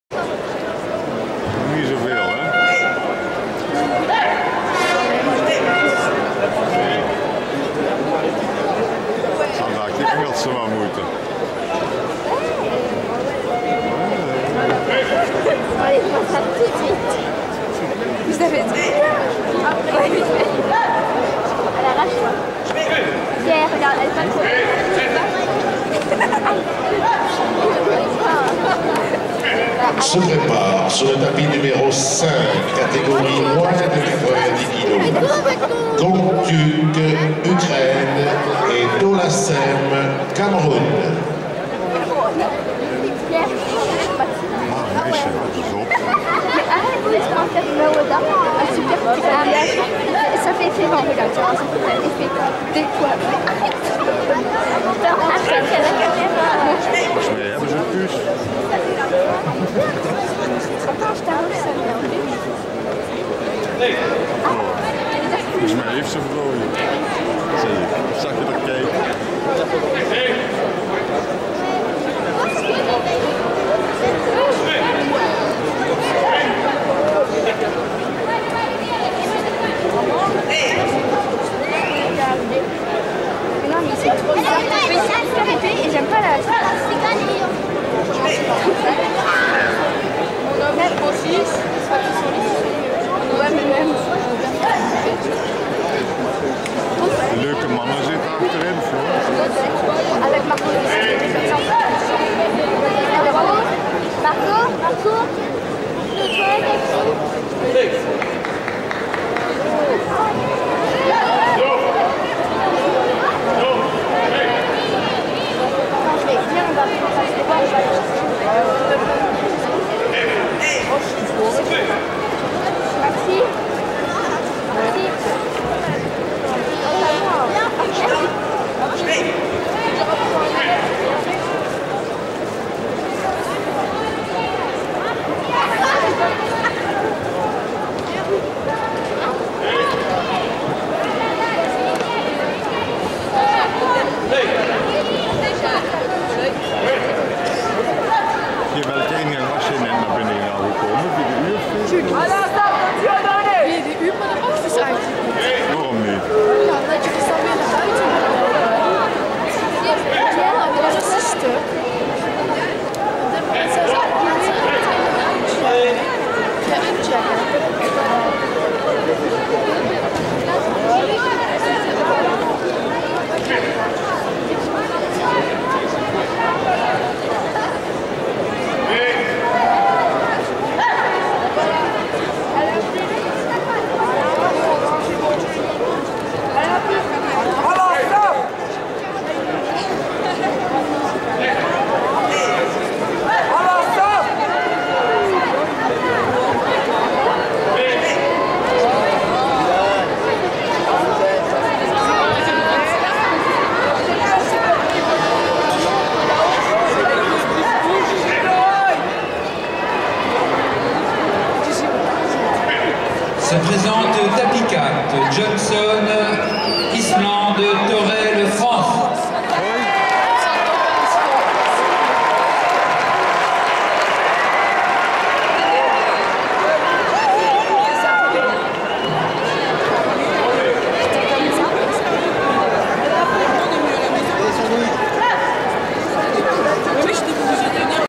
Judo 2012 Grand Slam Paris: Kaya (TUR) - Cutro-Kelly (USA) [+78kg]
Belkis Zehra Kaya (TUR) - Nina Cutro-Kelly (USA) [+78kg]
Judo 2012 Grand Slam Paris